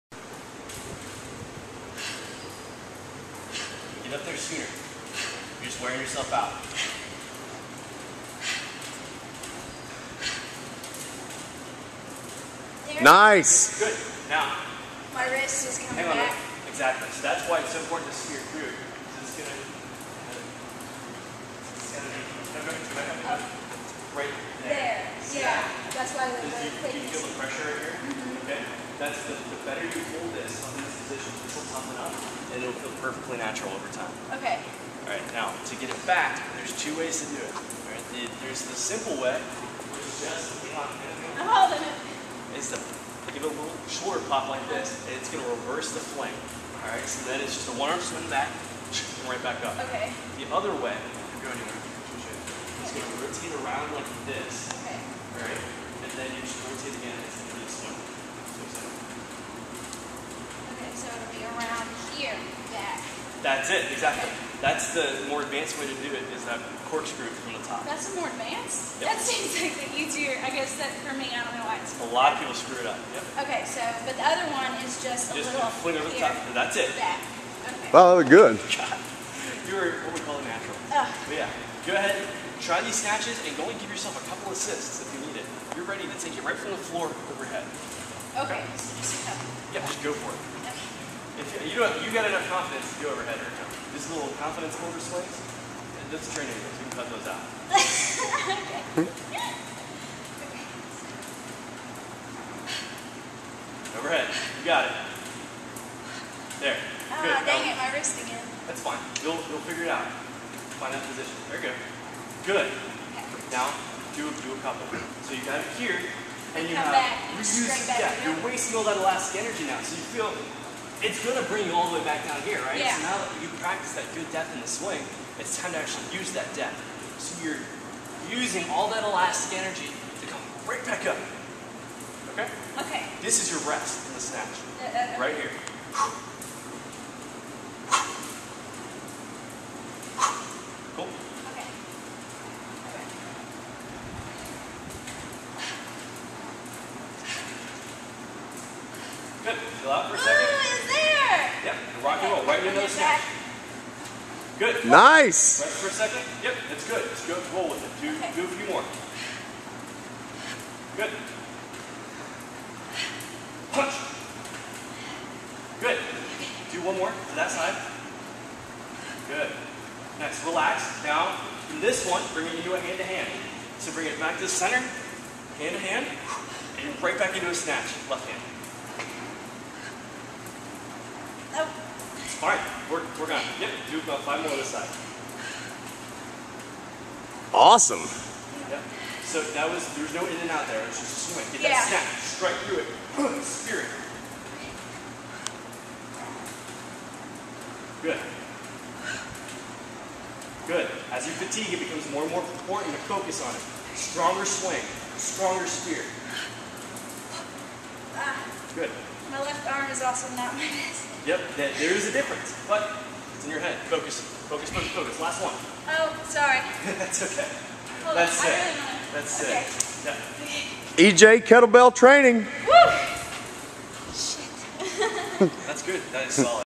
Crossfit Kettlebell 1-arm snatch
501 Crossfit trainer Elizabeth demos 1-arm kettlebell swings